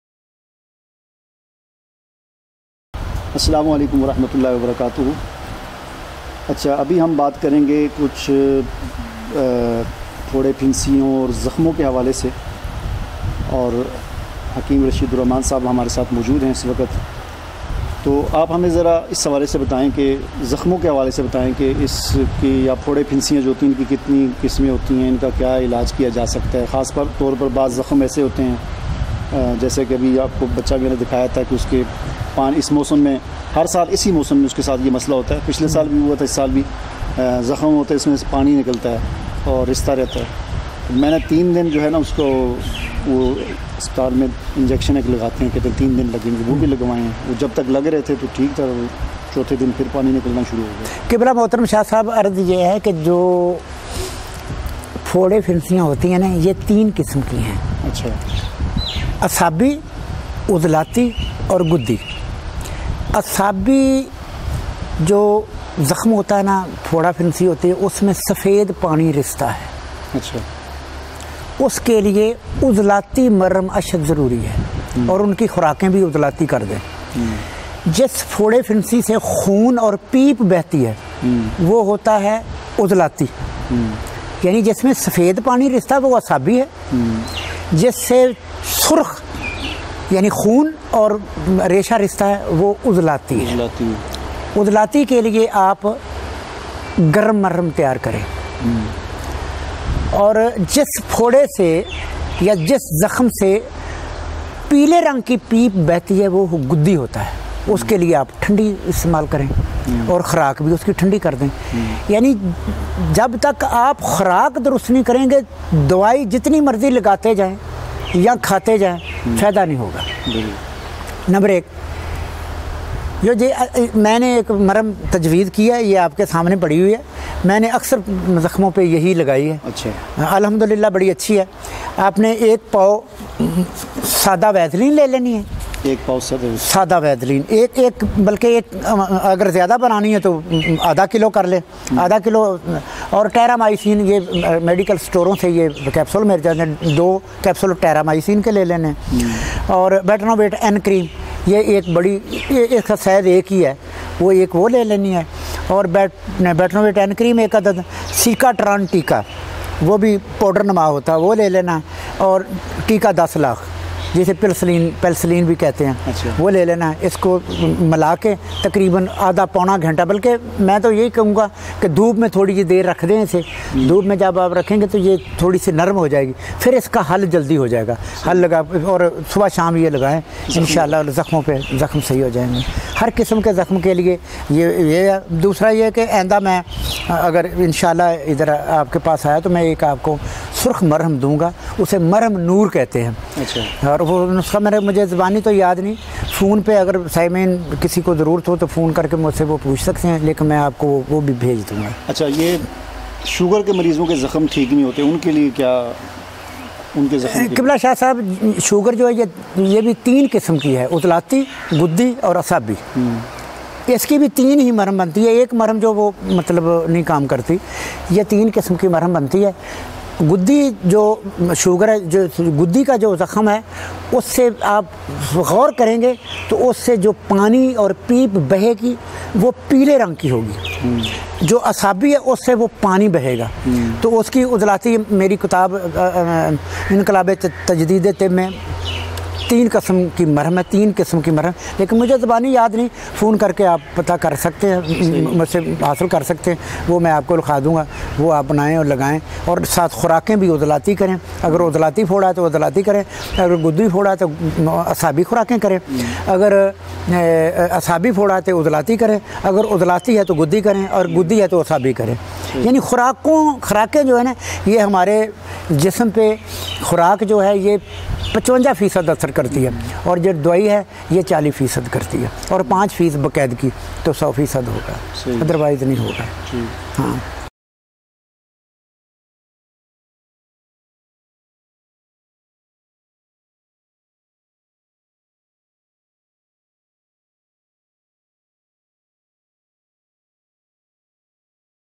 Wound Boil ► Video 4 ► زخموں کا مرہم ► फोड़ा घाव ►Hakeem Mian Raseed Ul Rahman ► Nukta Guidance
پھوڑے پھنسیوں اور زخموں کے لیے ایک بہترین مرہم خود اپنے گھر تیار کریں۔
Causes, Symptoms, Diagnosis, and Treatment Education